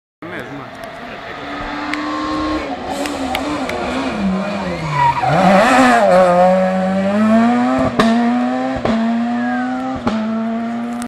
Bruno Magalhães em Peugeot 207 S2000 Mortágua 2008
Rally Mortágua 2008, prova do Campeonato Nacional de Rallys. Local: Cerdeirinha